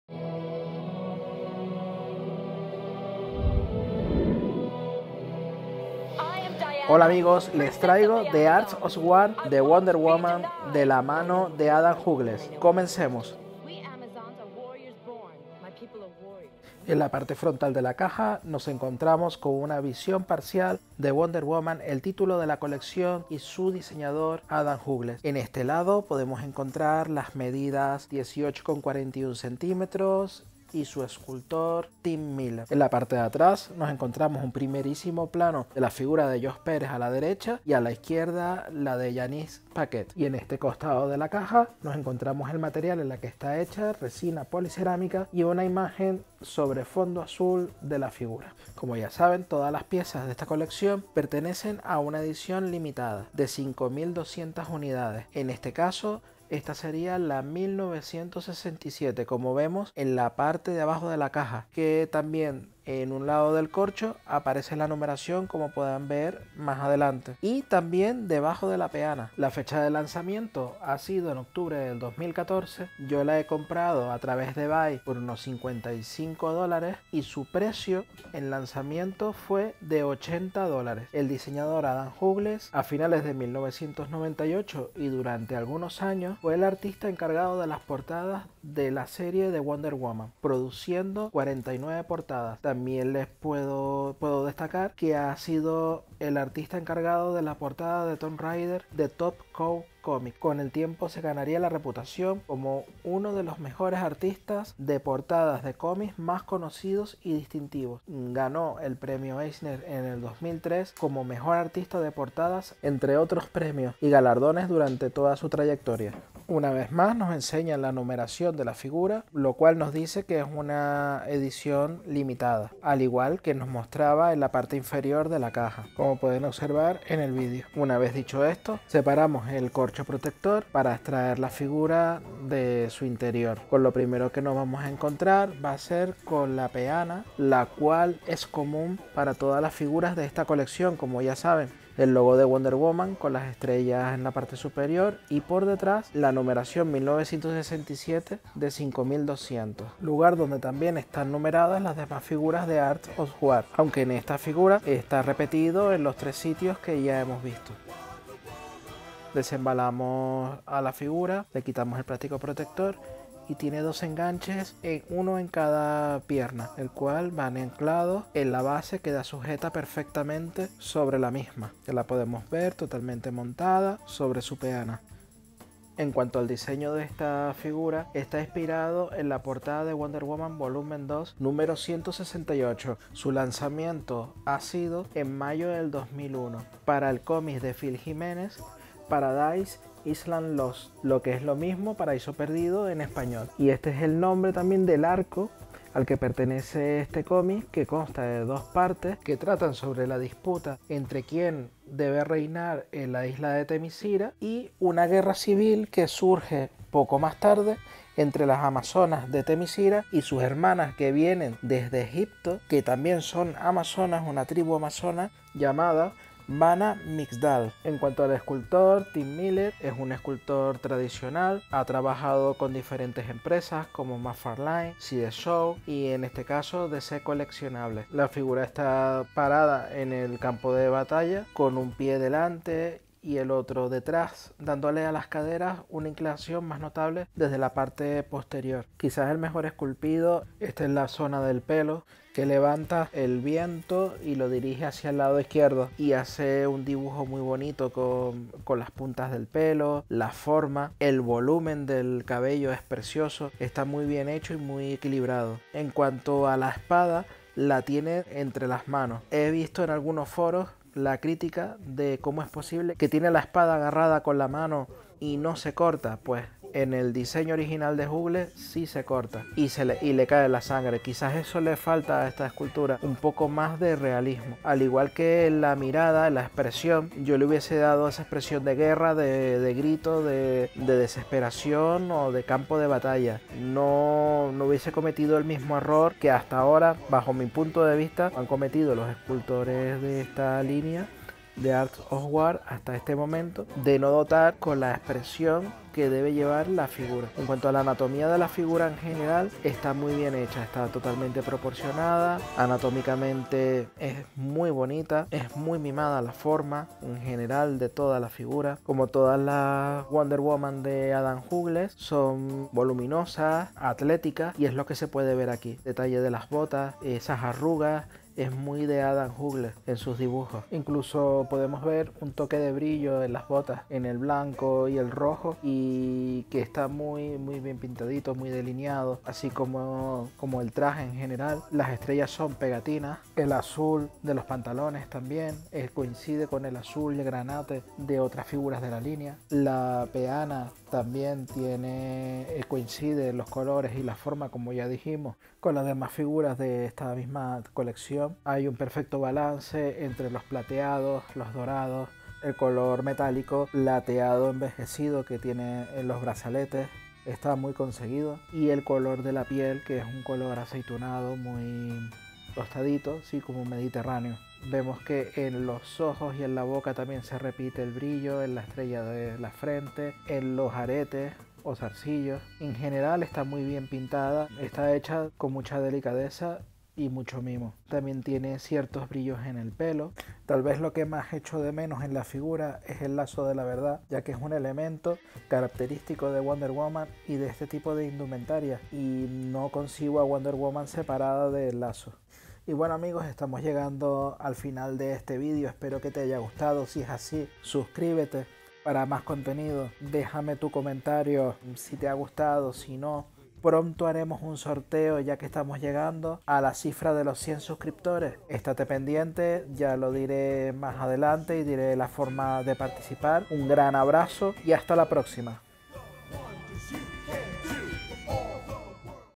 Unboxing Figura DC Wonder Woman: The Art of War Adam Hughes Statue Review en Español
Tercera figura de la colección The Art of War diseñada por Adam Hughes y esculpida por Tim Miller.

Gracias por vernos. y registrarte al Canal Oculto en Themyscira!!!